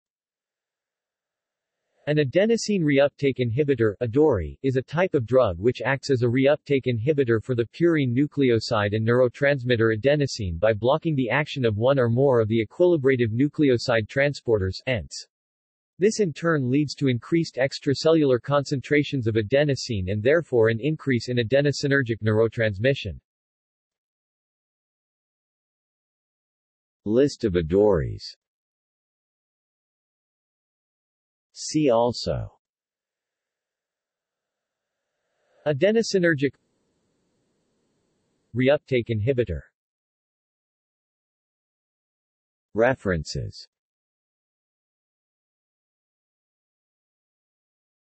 Adenosine reuptake inhibitor | Wikipedia audio article
This is an audio version of the Wikipedia Article:
Adenosine reuptake inhibitor

SUMMARY
An adenosine reuptake inhibitor (AdoRI) is a type of drug which acts as a reuptake inhibitor for the purine nucleoside and neurotransmitter adenosine by blocking the action of one or more of the equilibrative nucleoside transporters (ENTs). This in turn leads to increased extracellular concentrations of adenosine and therefore an increase in adenosinergic neurotransmission.